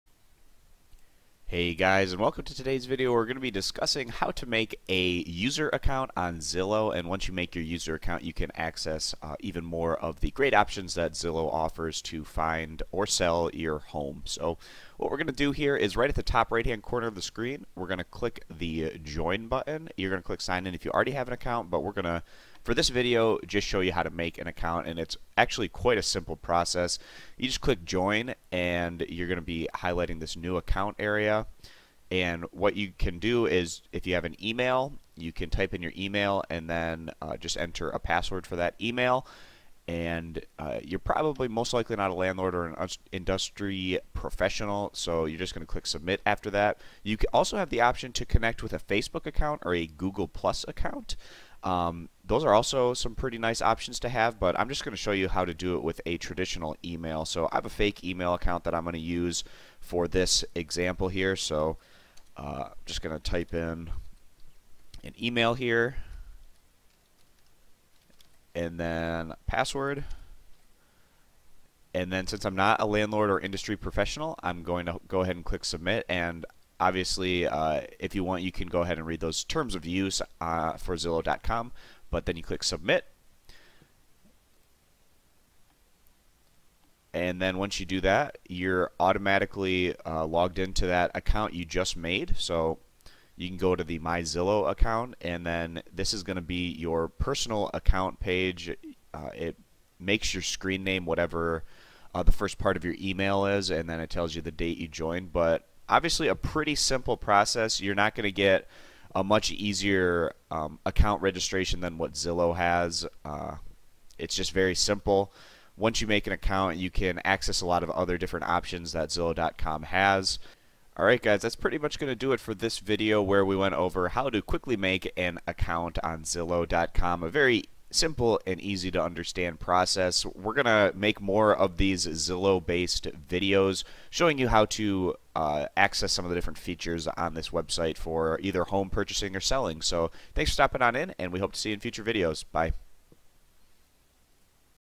How To Make Account on Zillow - Zillow.com Account Walkthrough
How To Use Zillow - How To Make Account on Zillow - Zillow.com Account Walkthrough

Hi guys, I'm making a series of videos dedicated to helping first time users on Zillow or just helping people that have questions on how to use the features on the site. This can be anything from how to make an account, search, filter, save homes, save searches, exc...You can access Zillow through the following